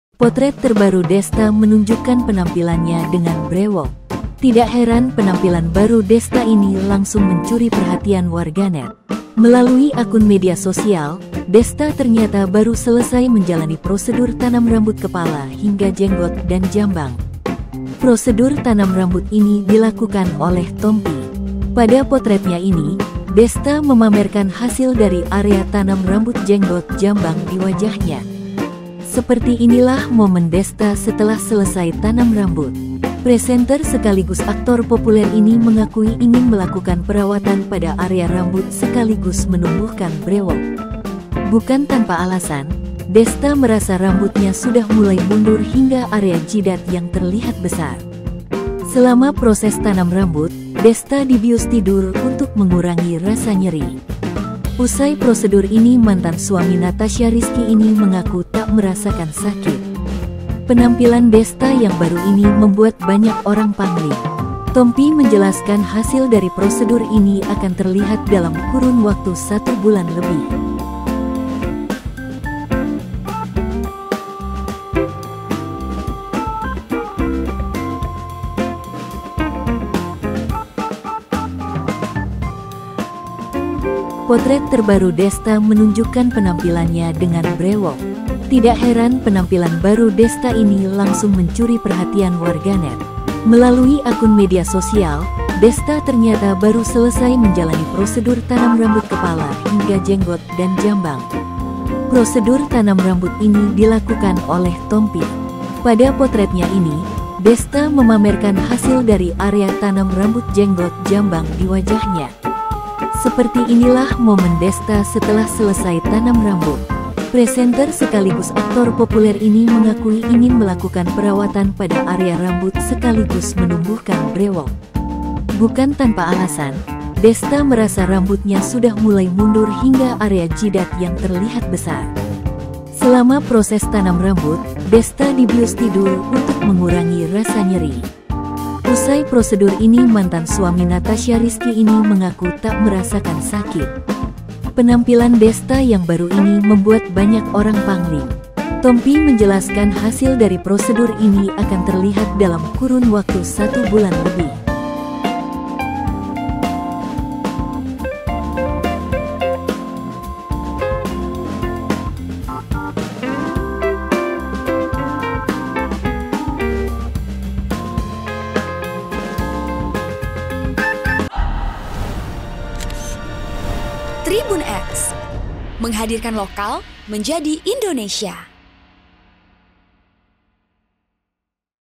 Potret Desta Jalani Transplantasi Rambut dan Jenggot, Langsung Pamer!
Potret terbaru Desta menunjukkan penampilannya dengan brewok. Tidak heran penampilan baru Desta ini langsung mencuri perhatian warganet.

Prosedur tanam rambut ini dilakukan oleh Tompi. Pada potretnya ini, Desta memamerkan hasil dari area tanam rambut jenggot jambang di wajahnya. Seperti inilah momen Desta setelah selesai tanam rambut.

Presenter sekaligus aktor populer ini mengakui ingin melakukan perawatan pada area rambut sekaligus menumbuhkan brewok. Bukan tanpa alasan, Desta merasa rambutnya sudah mulai mundur hingga area jidat yang terlihat besar.

Selama proses tanam rambut, Desta dibius tidur untuk mengurangi rasa nyeri. Usai prosedur ini mantan suami Natasha Rizky ini mengaku tak merasakan sakit.

Penampilan Desta yang baru ini membuat banyak orang pangling.

Tompi menjelaskan hasil dari prosedur ini akan terlihat dalam kurun waktu 1 bulan lebih.

Program: -
Host: -
Editor Video: Tri Suryo
Uploader: -